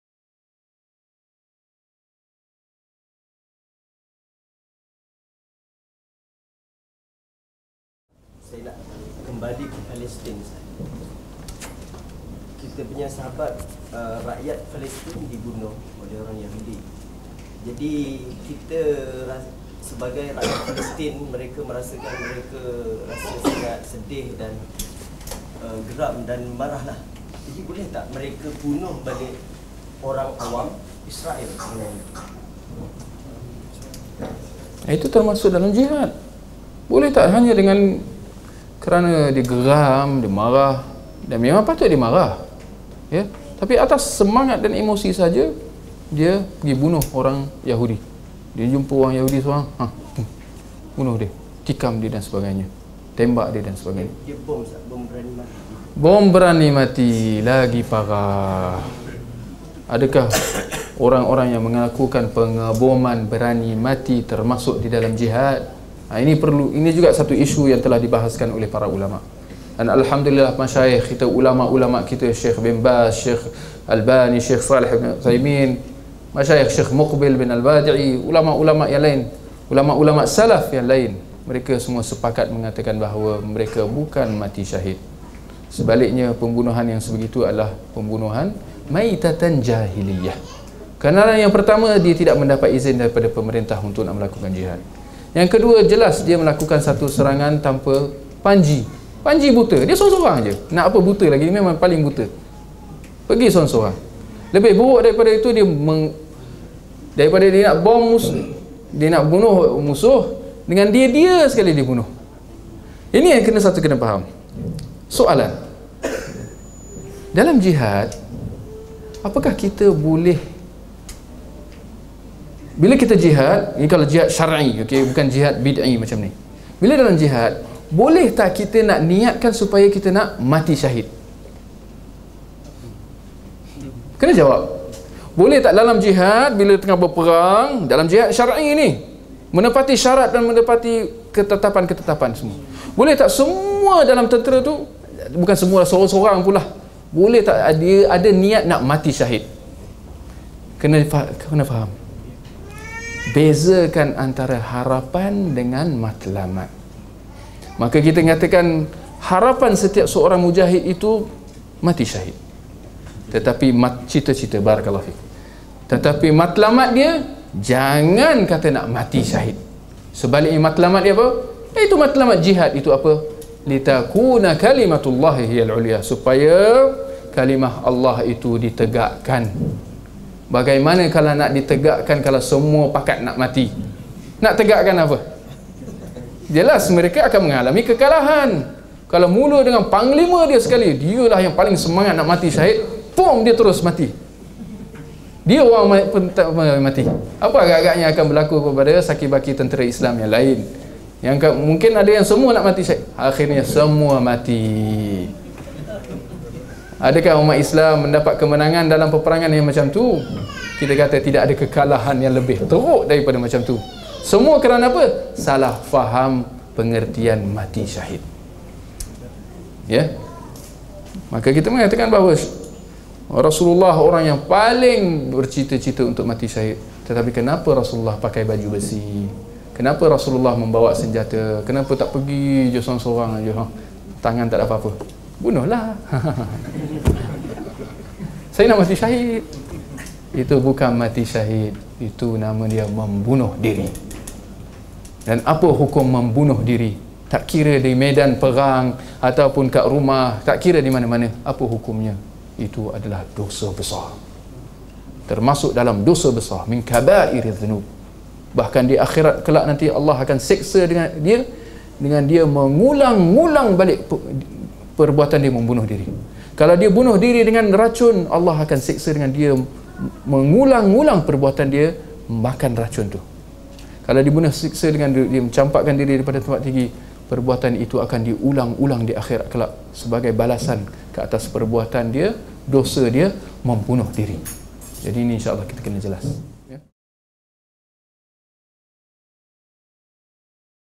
[PART 8] Ustaz Idris Sulaiman - Pengeboman Berani Mati, Jihad dan Mati Syahid
"Palestin : Antara Jihad dan Hijrah?"
Penceramah : Ustaz Idris Sulaiman
Tarikh : 4 Disember 2012 (Selasa)

SESI 2 (Soal Jawab)